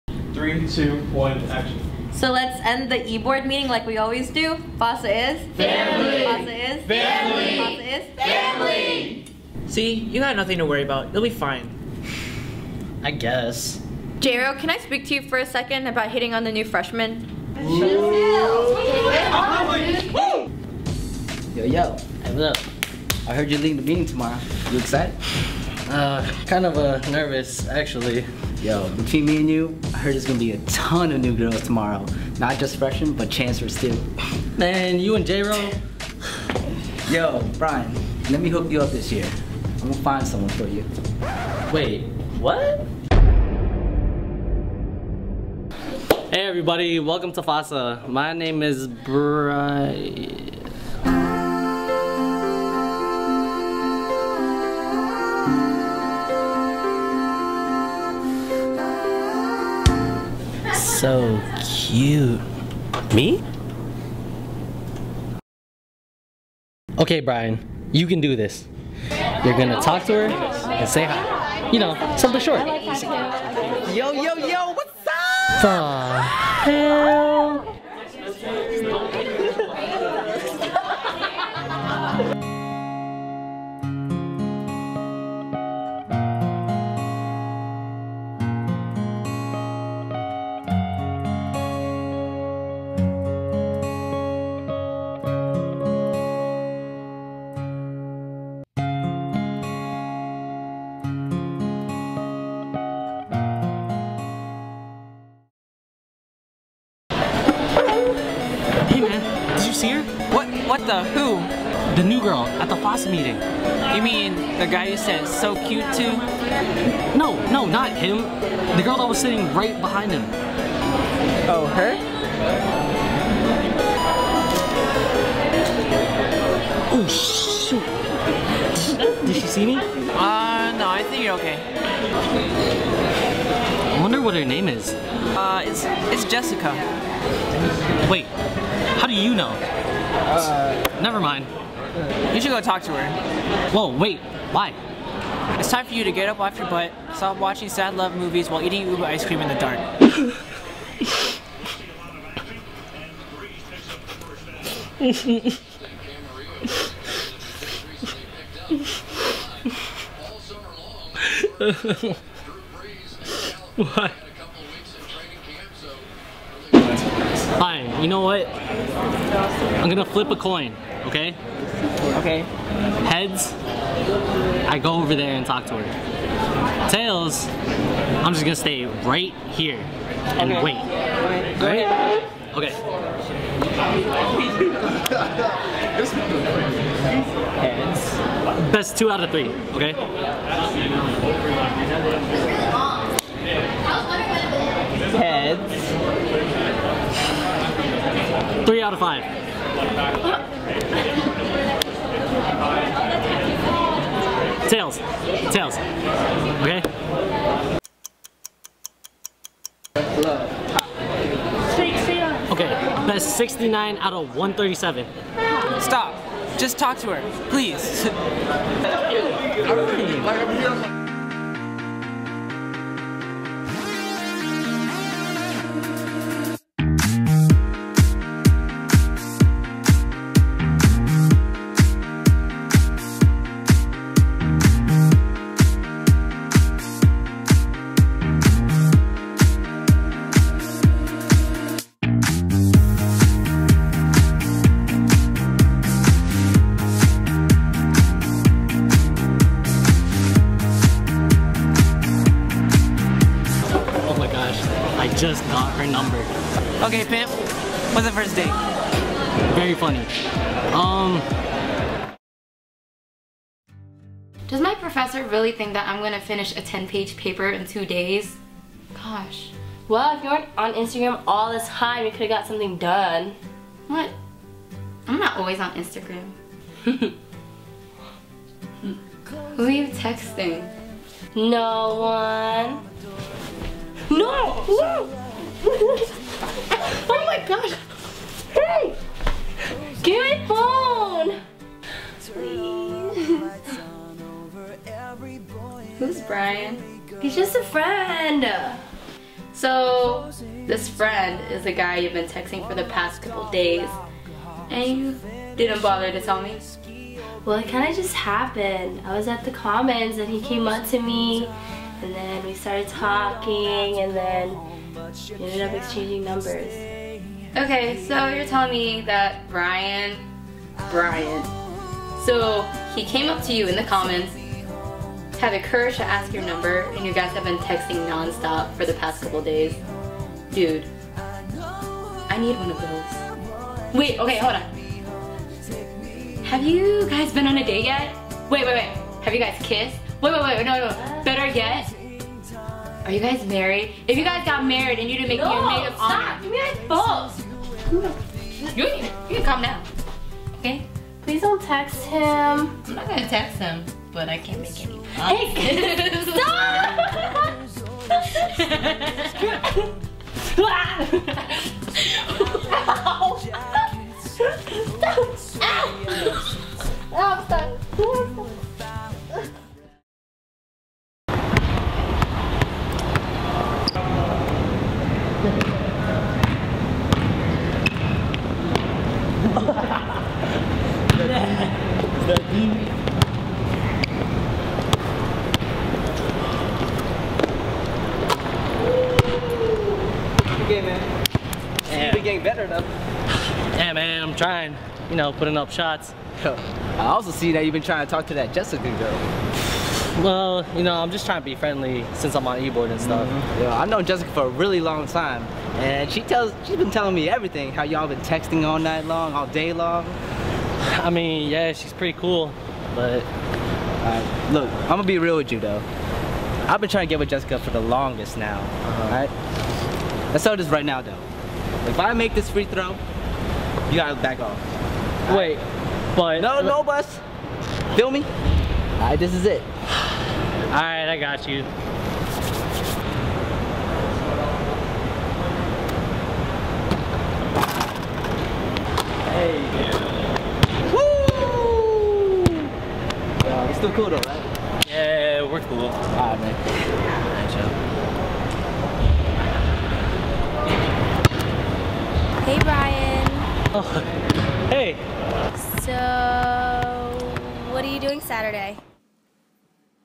Taking A Chance Episode 2
I'm apart of a Filipino-American group at my college and since my major deals with art, I decided to write this series to promote our semi-formal! It's also my first time writing, directing, camera operating, and editing something this big hahaha

I wanted to upload it on here to share it with you guys!

UMBC FASA presents Taking a Chance. FASA's first youtube video series that include current FASA (2012-13) members Carlo Canasa (Bryan), Jahziel Paulines (Jessica), Johnross Famisan (J-Ro), Gretchen Quiteles (Jenny) & Nathan Nazareno (Alex).

Written by Jason Garcia
Directed by Jason Garcia & Joseph Rigoroso